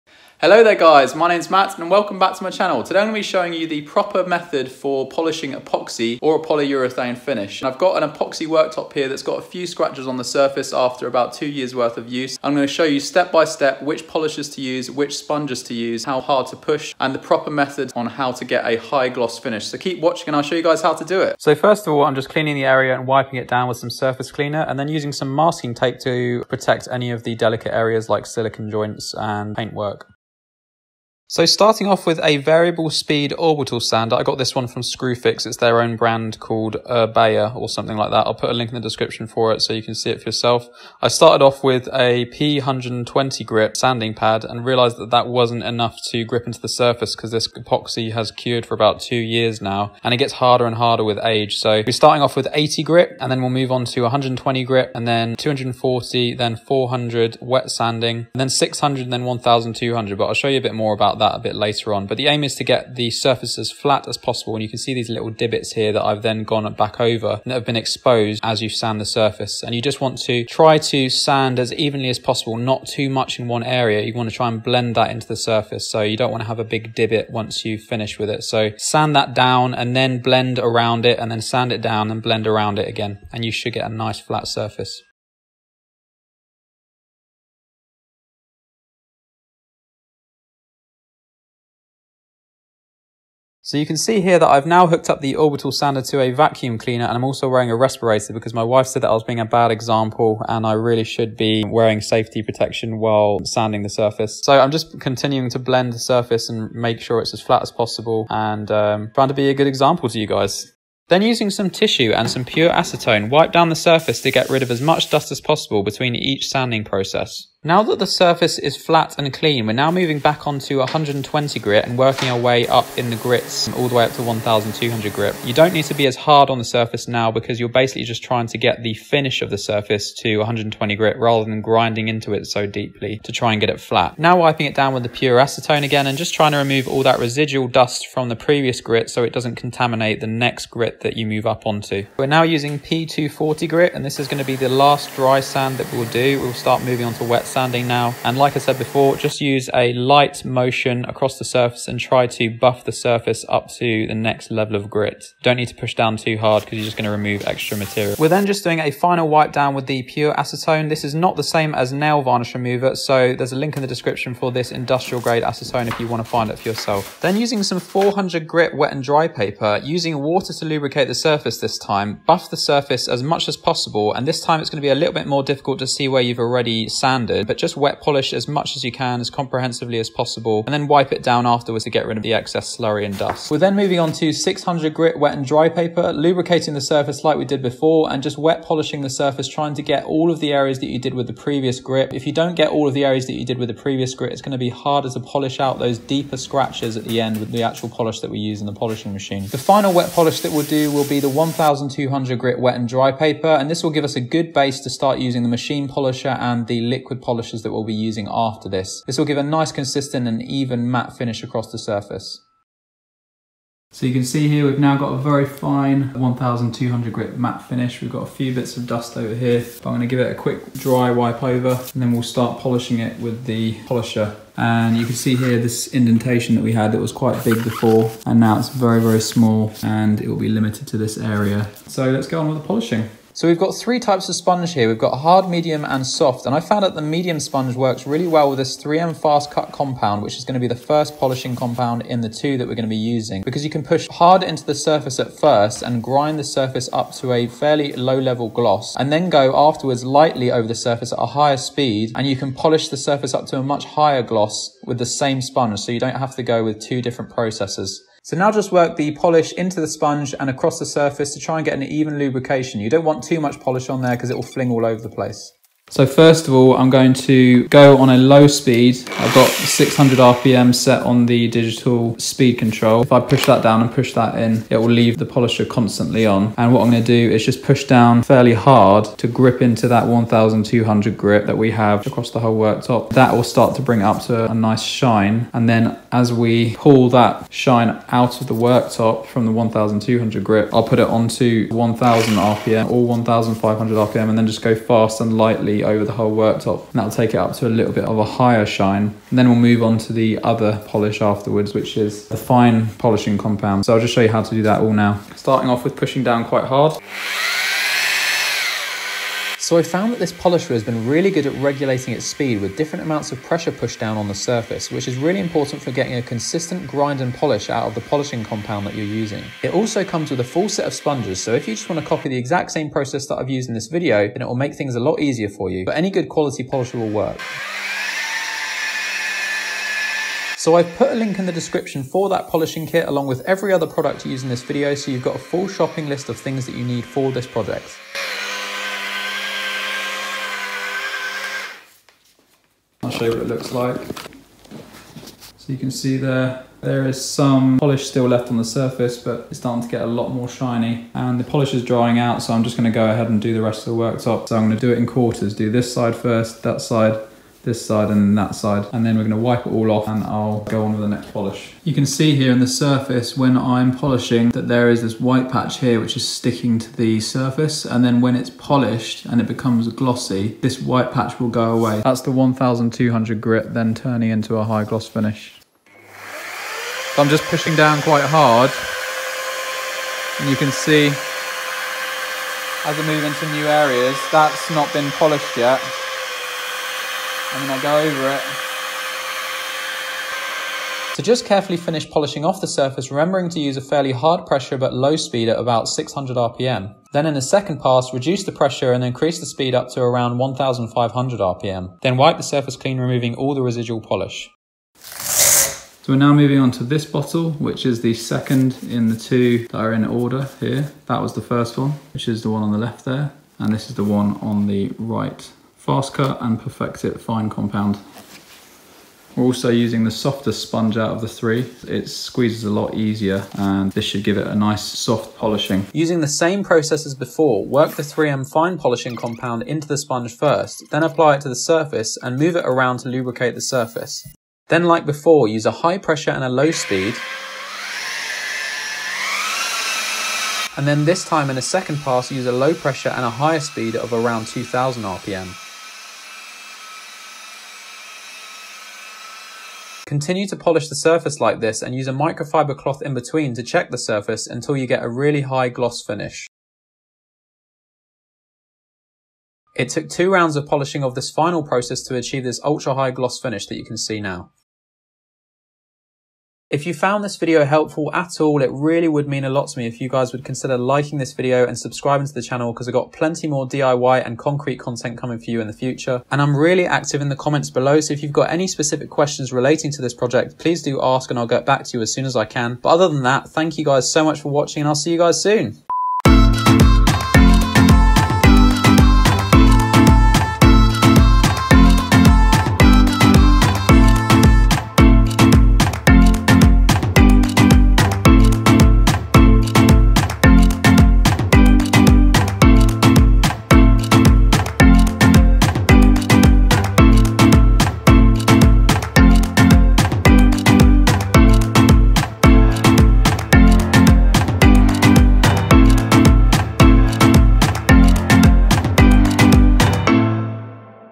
FASTEST METHOD! How to Sand & Polish Epoxy Resin
This is the fastest and most aggressive method for polishing an epoxy resin worktop / countertop . Tool & Products Lists: Machine Polisher and Sponges: UK

Wet and Dry Sand Paper Kit: UK

3M 09374 Fast Cut Polishing Compound: UK

3M 09375 Perfect-it III Fine Compound: UK

I’ve got loads more DIY videos on my channel and lots more to come in the future.

Best regards, Matt